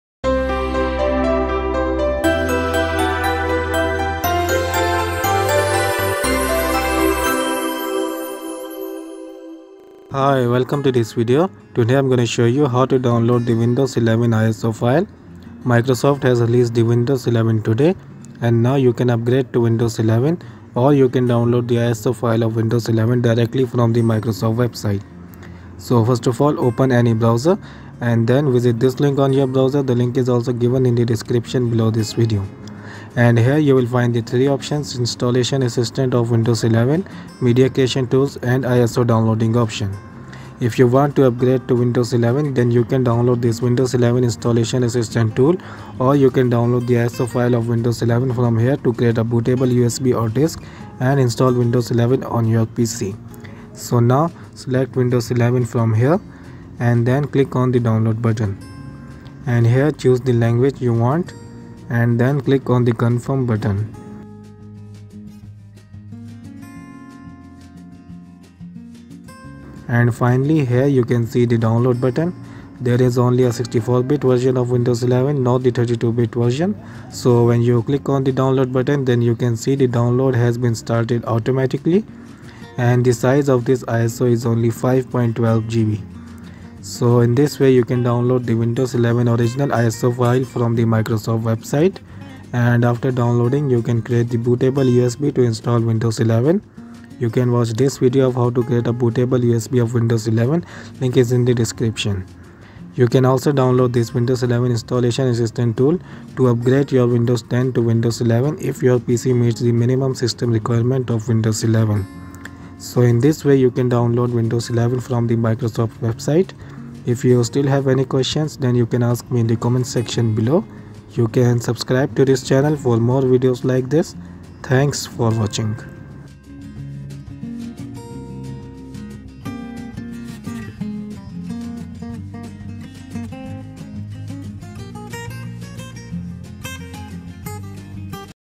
Windows 11 ISO | How to Get Windows 11 Original ISO File From Microsoft Website
This video is about how to get the windows 11 iso file. You can get windows 11 iso free from Microsoft for installation.